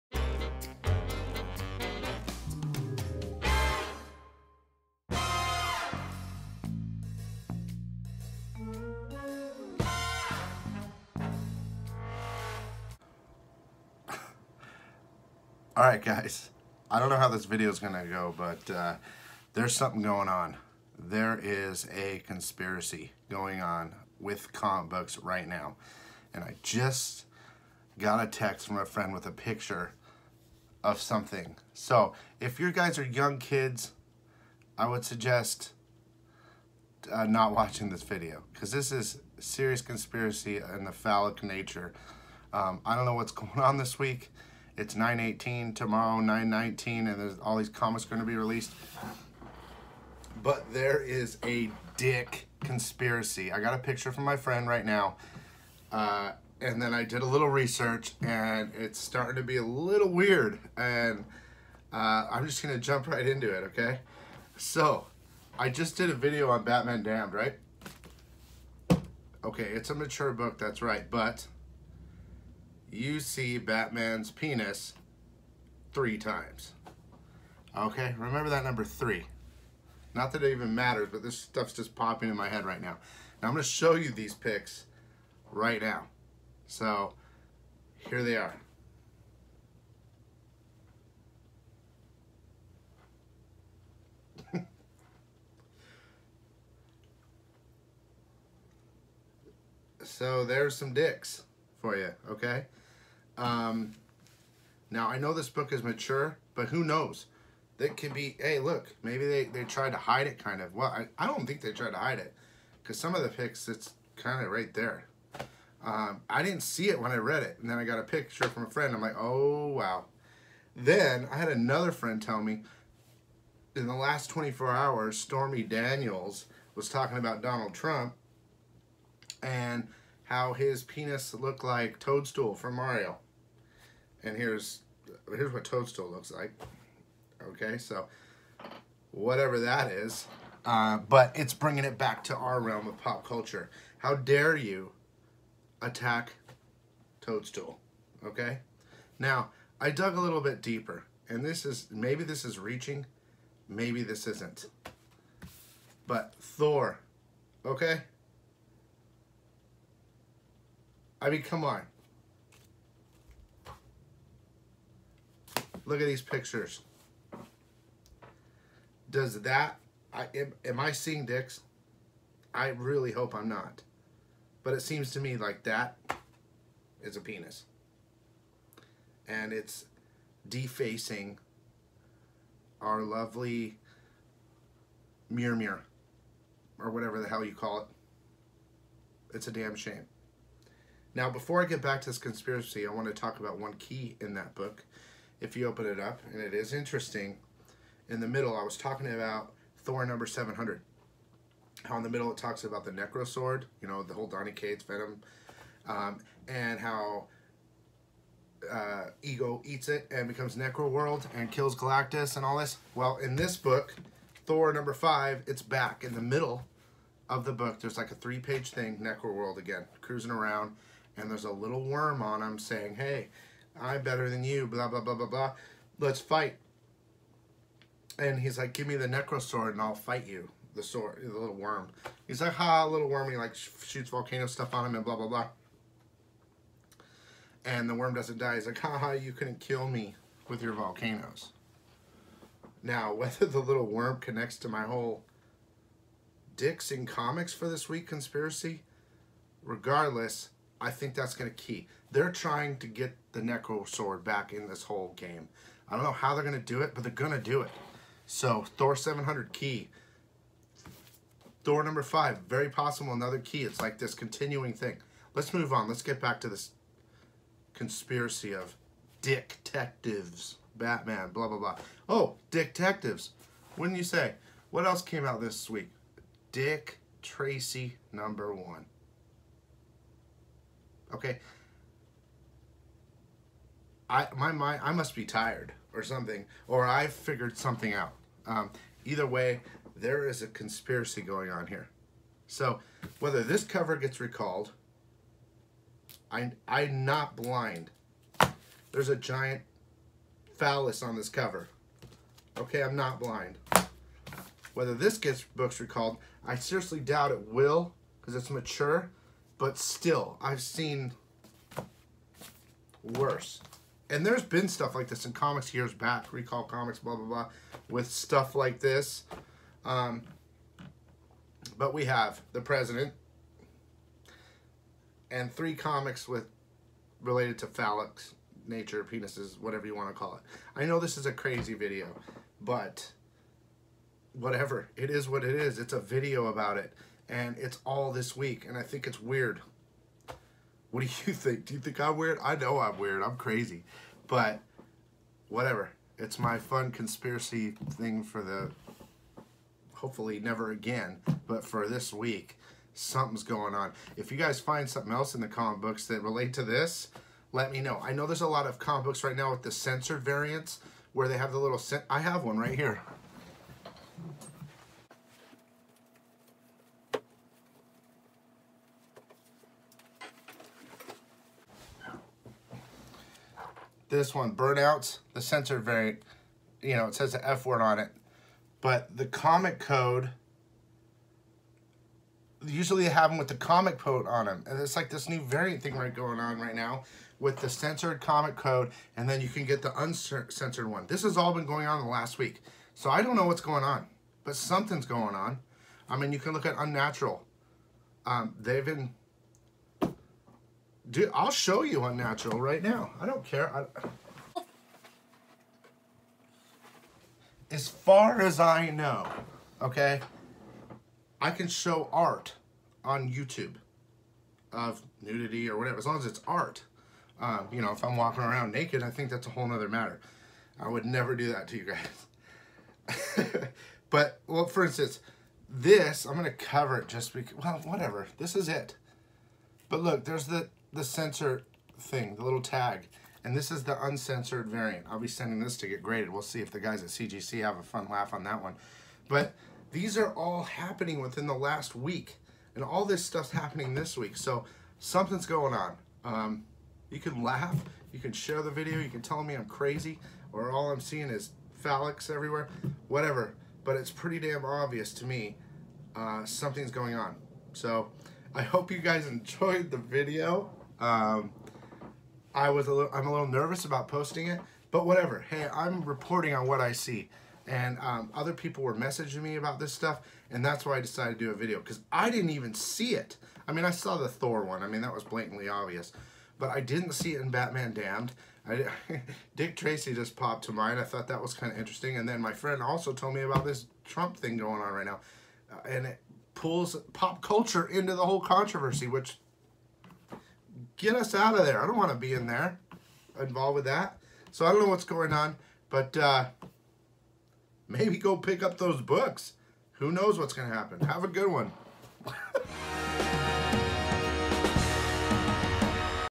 BREAKING NEWS "MAJOR COMIC BOOK CONSPIRACY" 9/19/18 CONTROVERSIAL RECALLS? DICK'S IN COMICS?!
Every major comic book publisher involved as well as the President and Pop Culture.  What is happening to comics?  To much evidence is piling up.  Possible recalls on comics.  What in the world is happening to my beloved comic book industry???  Let's find an answer to this madness.  Thanks